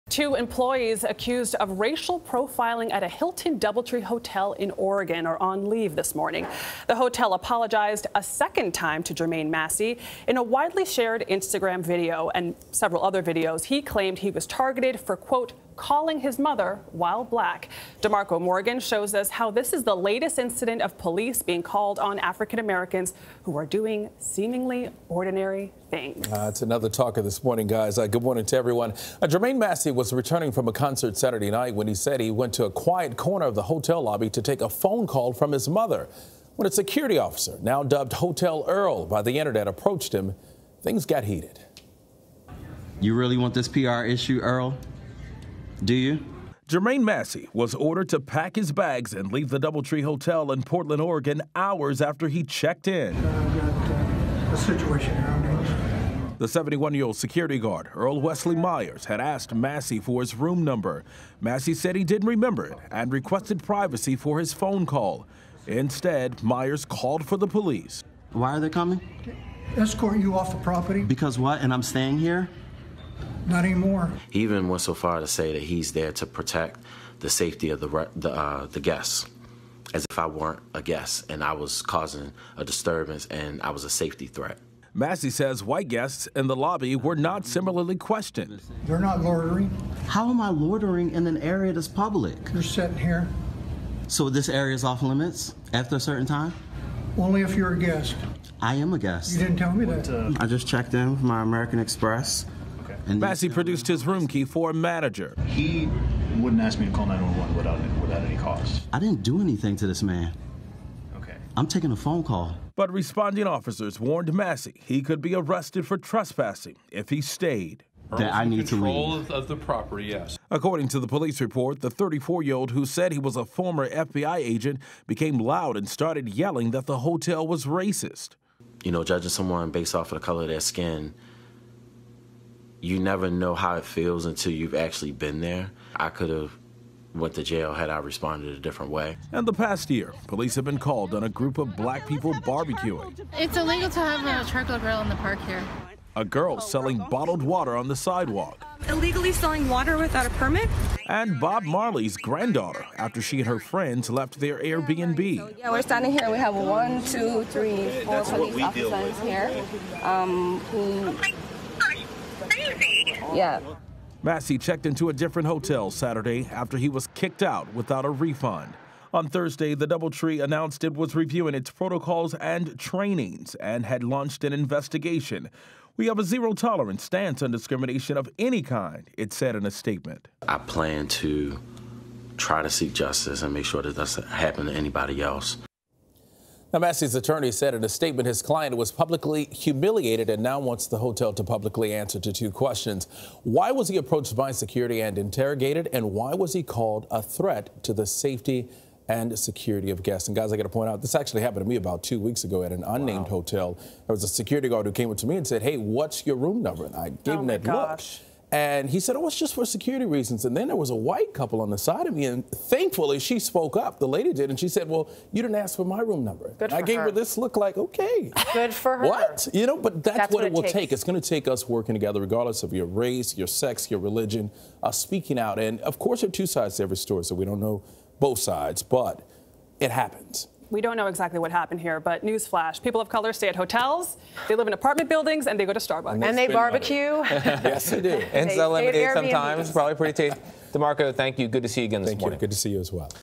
"Hotel Earl": Employees who called cops on black guest put on leave
Two employees accused of racially profiling a black guest in the lobby of a Hilton DoubleTree Hotel in Oregon are on leave Friday morning. The hotel apologized a second time to Jermaine Massey, who in widely shared Instagram videos claimed he was targeted for "calling his mother while black." DeMarco Morgan reports.